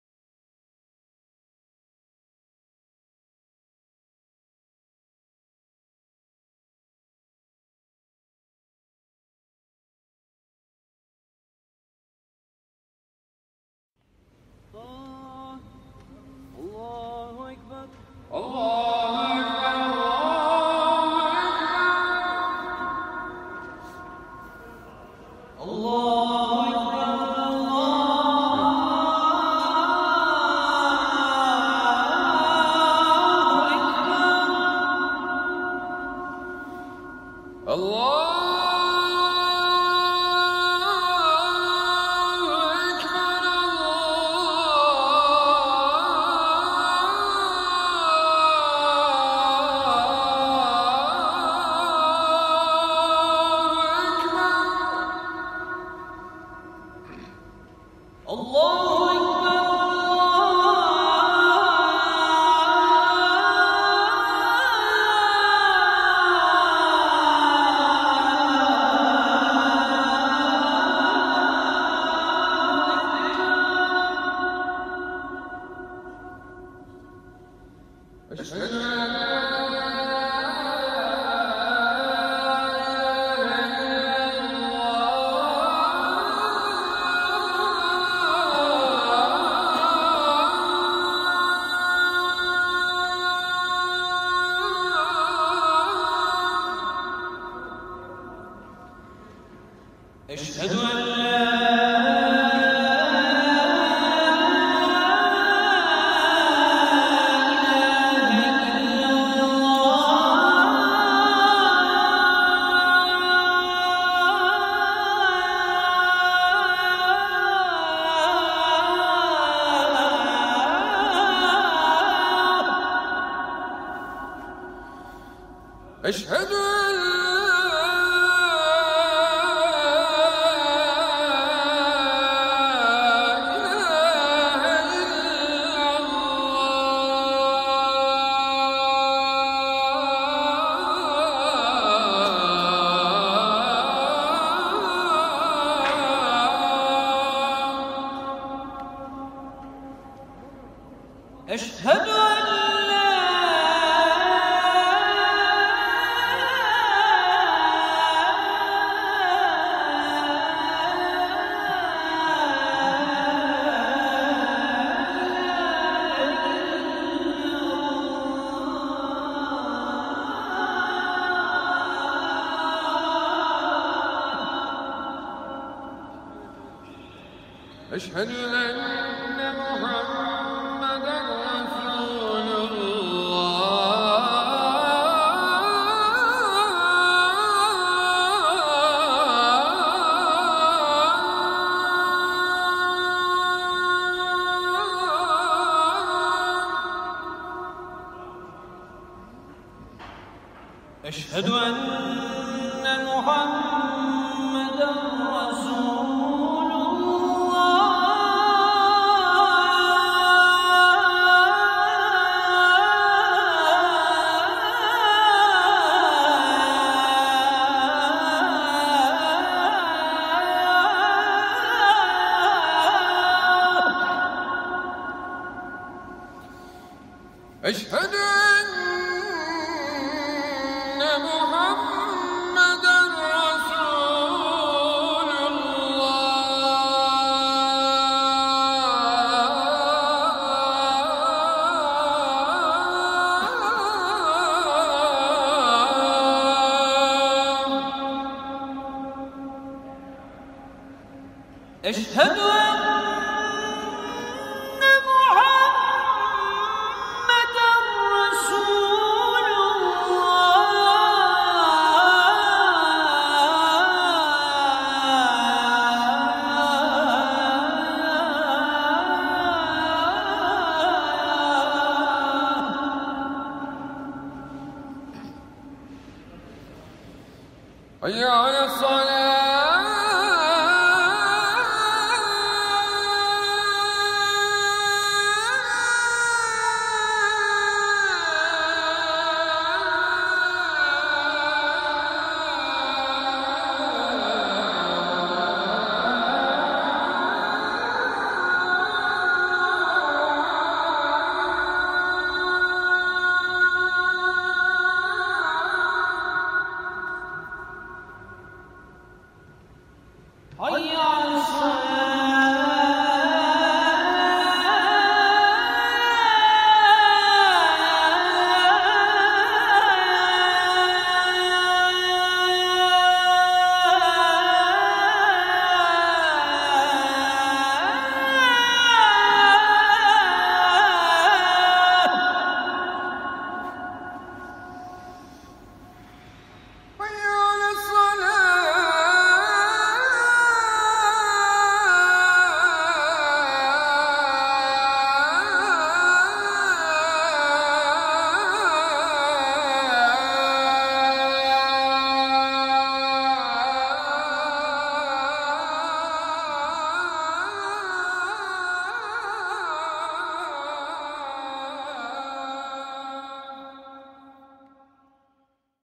Discover Al-Andalus: The Great Mosque of Cordoba
A small Muslim community is putting roots down in the historic city of Cordoba. Come and see for yourself. Come and support us.